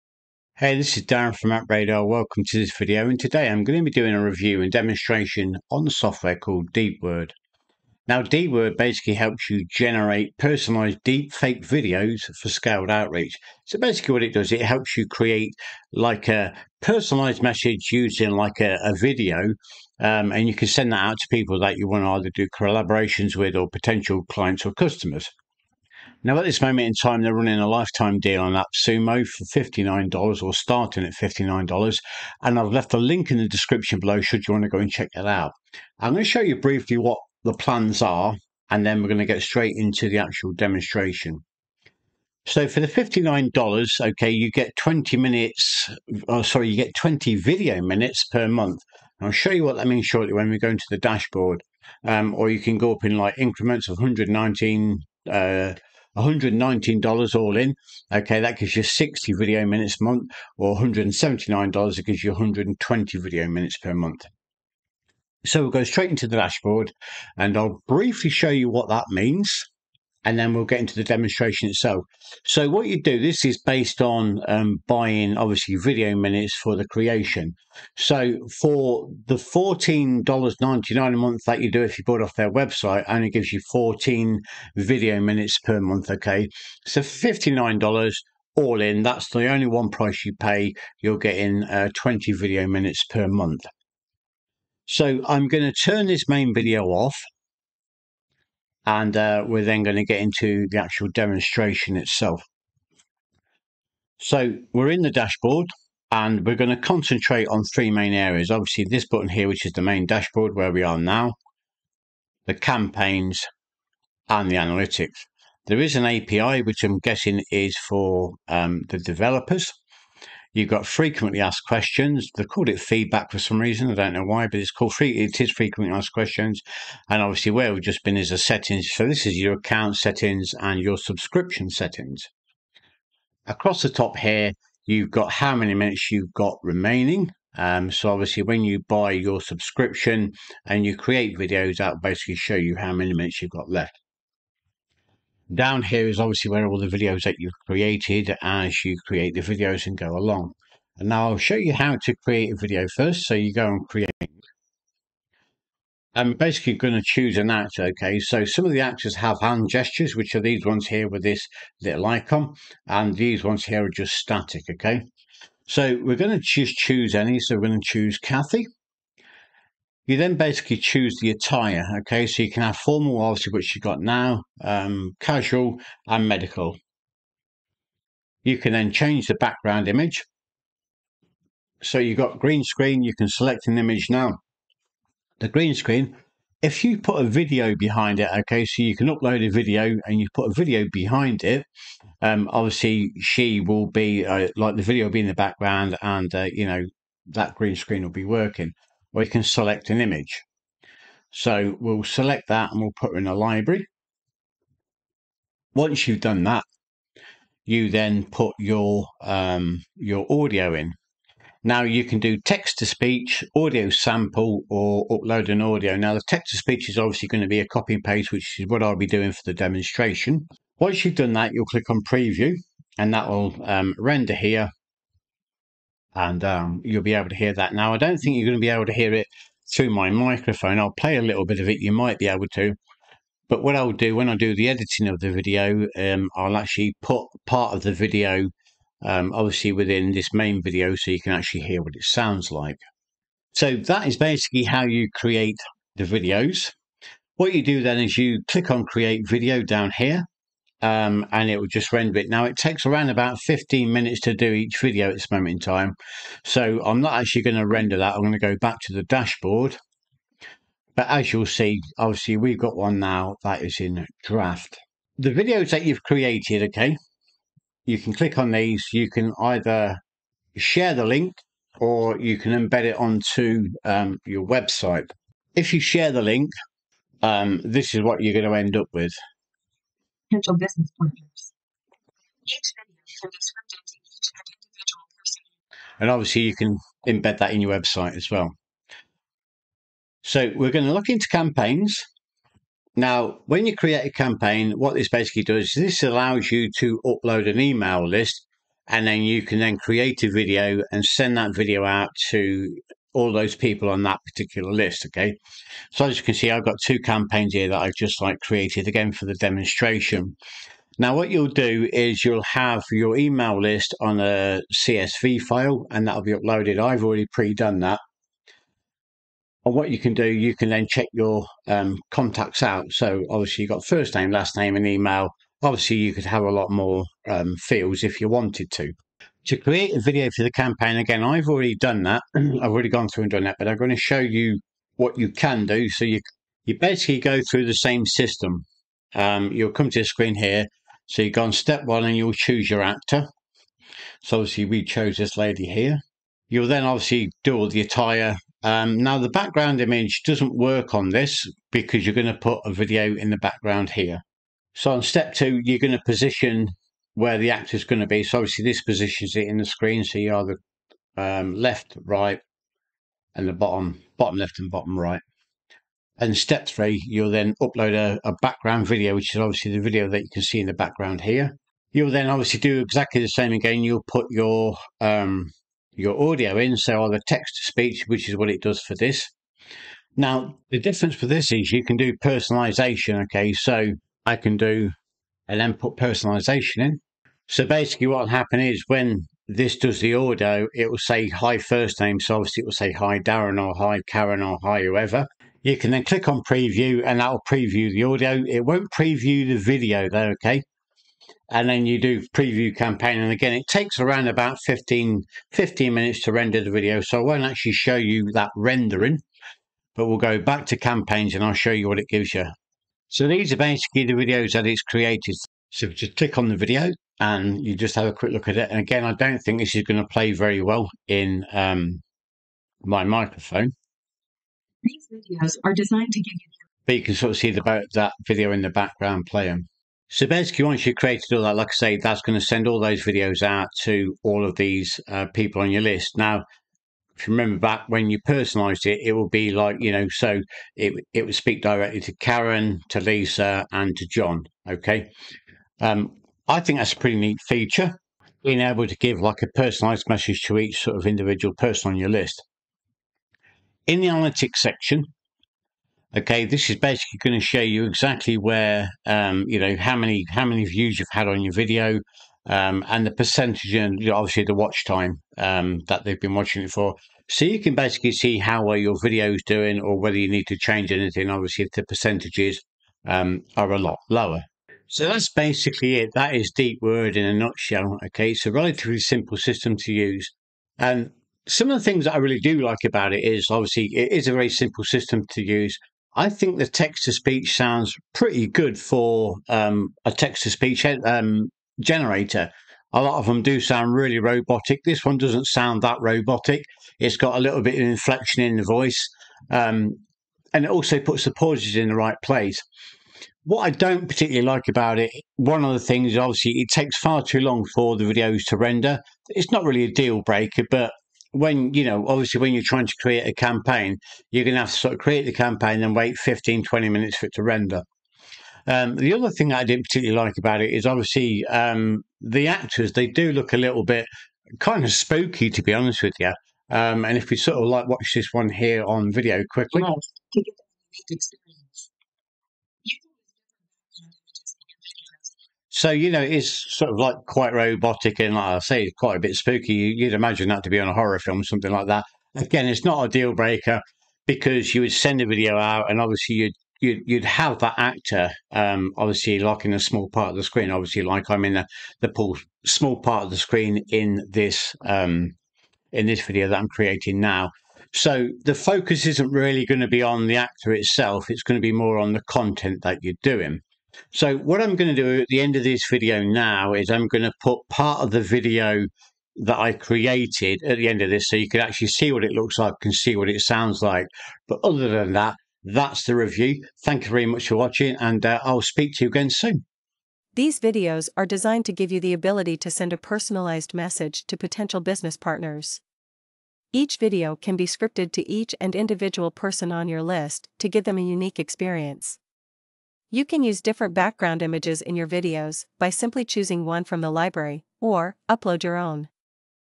Deepword Review - Can the Deepword AI REALLY Capture Prospects Attention?
Deepword Review: In this deepword review and demo video, I will show
you behind the scenes of the software so you can make an informed
decision on whether deepword is a good fit for your business.

Jump to a section:
00:00 Deepword Review

With DeepWord, you can create deepfake videos of people talking—just
type what you want them to say, or upload an audio file of what you want
to hear.

Choose one of DeepWord’s video actors or create your own for scaled use.

In less than 15 minutes, you’ll be able to generate a deepfake video
that can be downloaded, shared, or translated.

DeepWord helps you save time and money personalizing videos for
individual sales and marketing contacts at scale.

You can upload CSV files from any CRM to instantly customize videos for
certain contacts with merge tags.

Whether you’re in education, sales, or marketing, you’ll be able to
integrate DeepWord into your favorite system for seamless distribution.

Make the right visual impression by choosing where to overlay your
generated video on custom backgrounds.